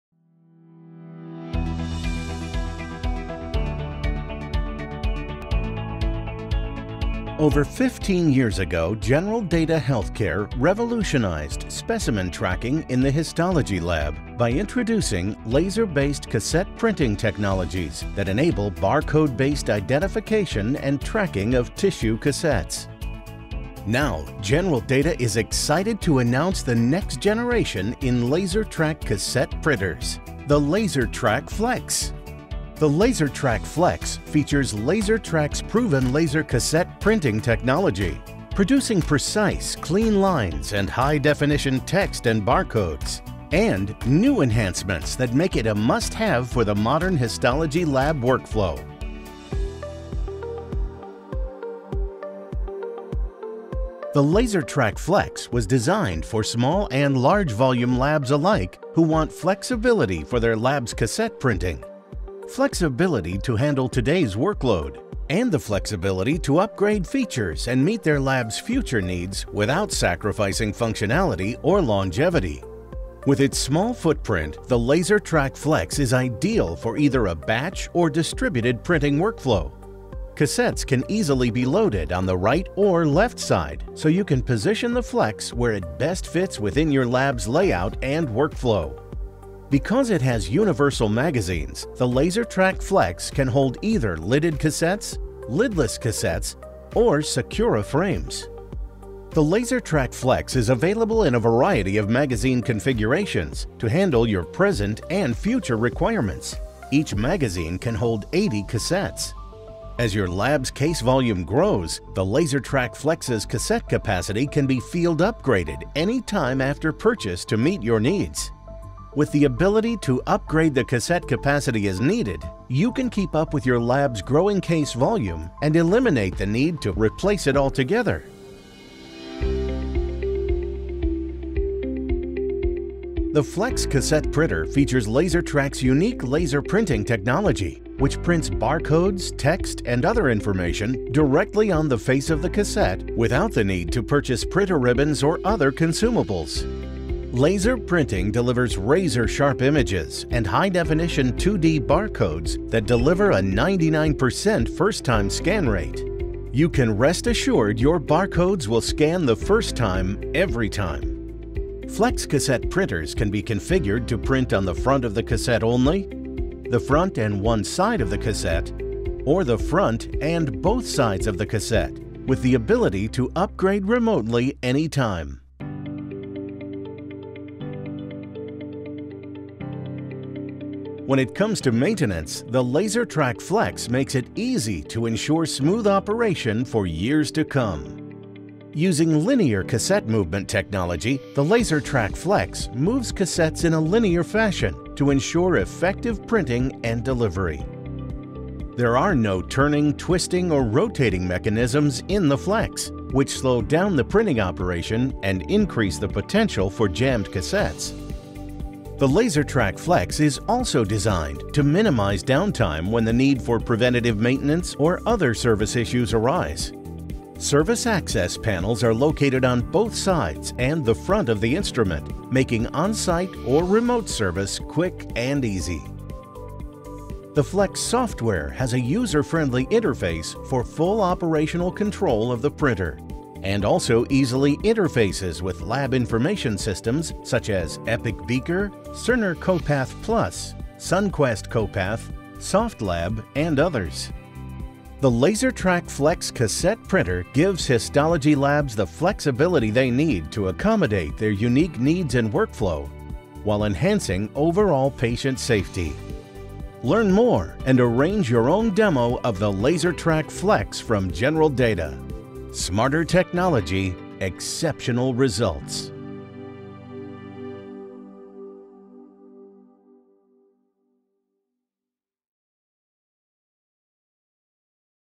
See The LaserTrack Flex Cassette Printer For Histology
The LaserTrack™ FLEX™ Cassette Printer is a revolutionary new concept for cassette printing in histology labs.  Instead of being locked into a set configuration for your lab’s cassette printers, the FLEX offers different configuration options that can be customized for your current needs.  In addition, the FLEX can be upgraded any time after purchase to meet your future needs without sacrificing functionality or replacing the printer entirely.